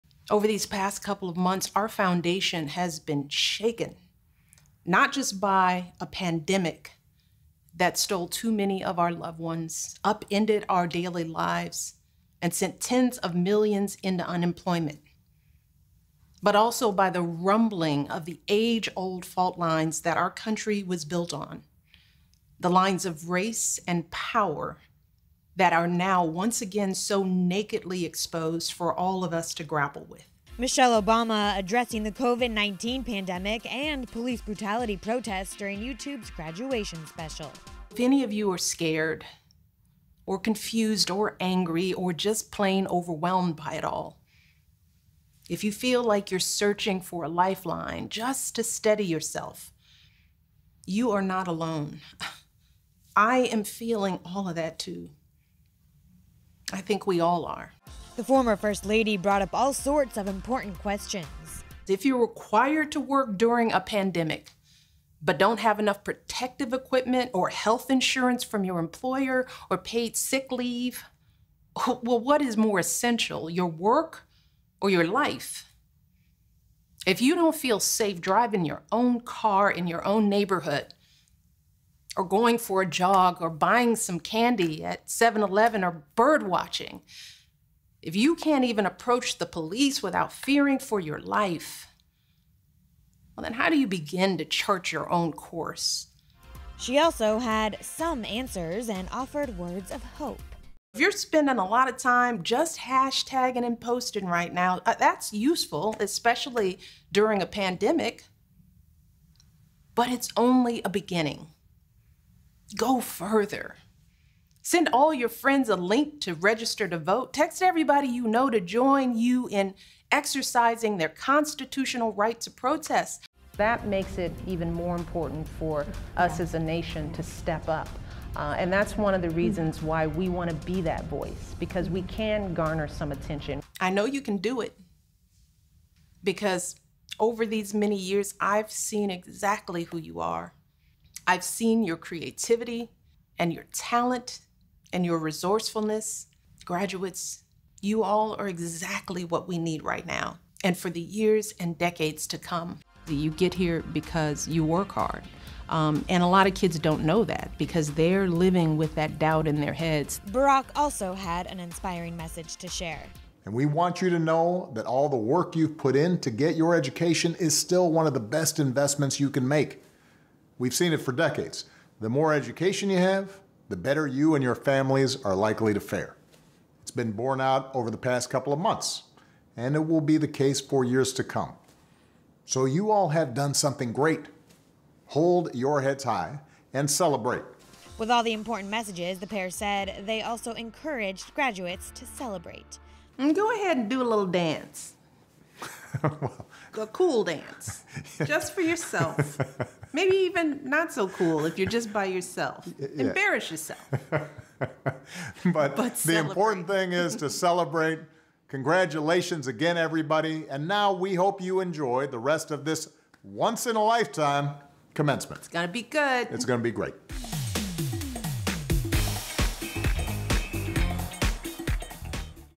Watch Michelle and Barack Obama’s Message to 2020 Graduates
The Obamas joined YouTube’s virtual graduation special to deliver some encouraging words to this year’s graduates.